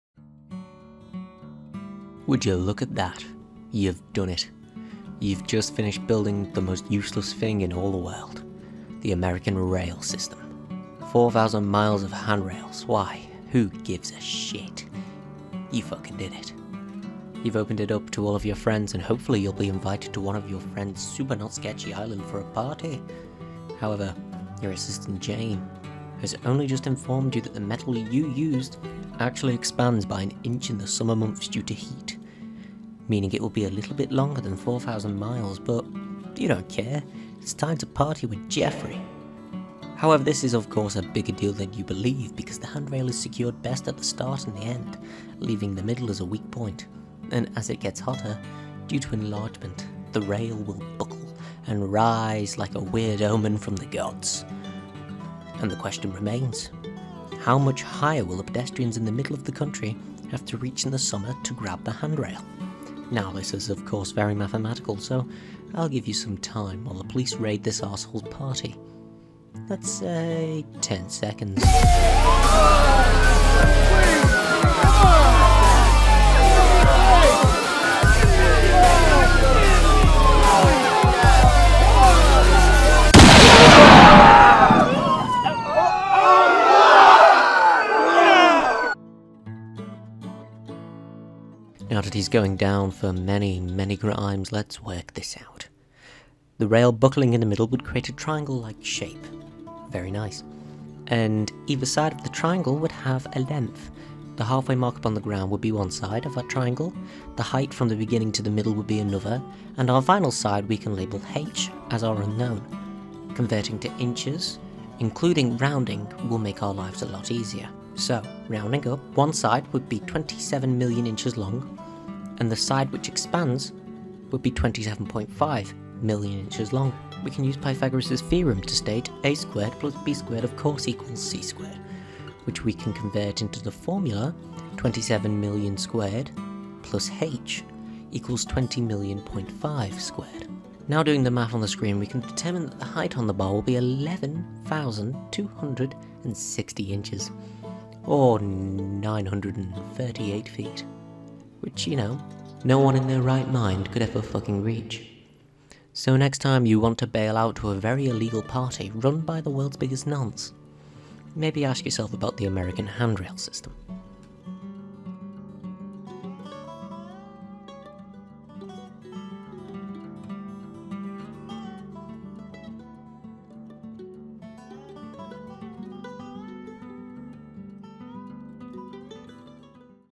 Are You Flummoxed? Episode 5: Off The Rails
The American Rail Trail. 4,000 miles of handrails,  You've opened it up to all your friends however, your assistant, Jane, has only just informed you that the metal you used actually expands by an inch over the summer months due to heat, meaning it will be a bit longer than 4,000 miles. The handrail is secured best at the start and the end leaving the middle as a weak point and as it gets hotter, due to the enlargement of the rails, it buckles and rises. Now the question remains: How much higher will pedestrians in the middle of the country have to reach in summer to grab the handrail?